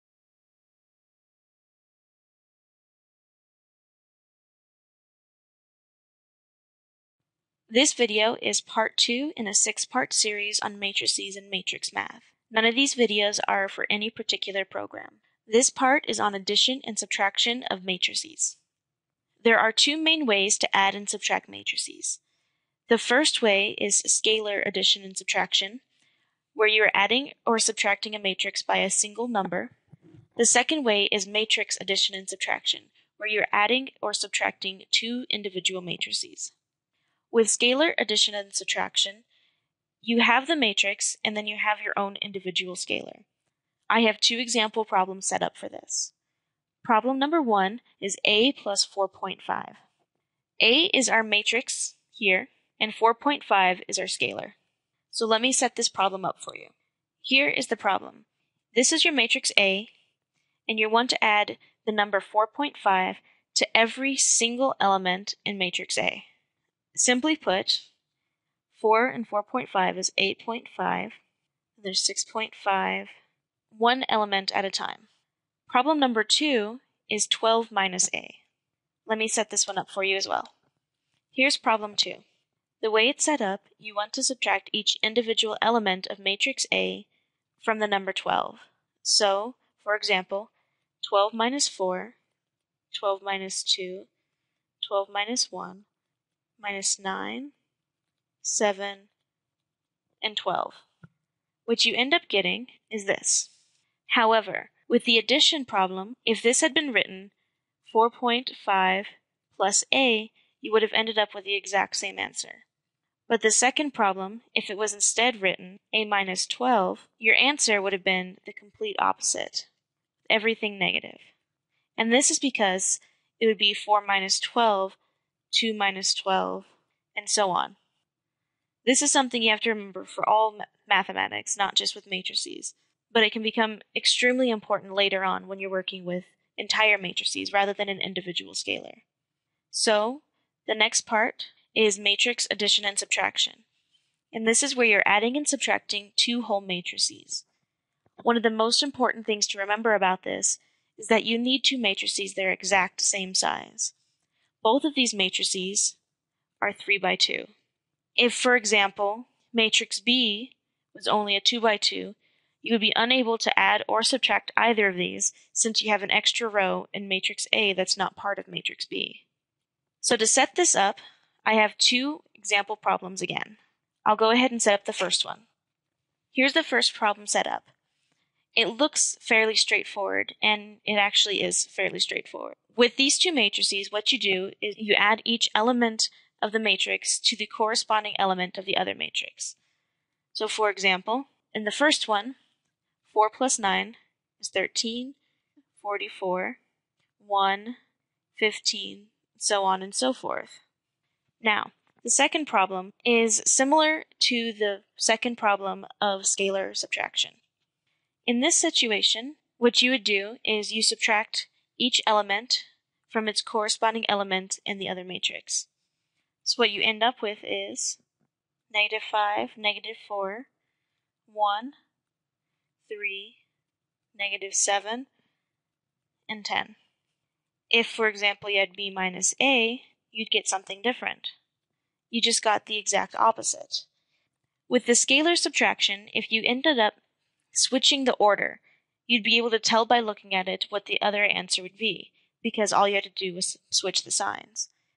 Matrix Addition and Subtraction
Explains elemental addition and subtraction of matrices. Part 2 of the matrix math series. Prepared at the University of Colorado Boulder Department of Chemical and Biological Engineering.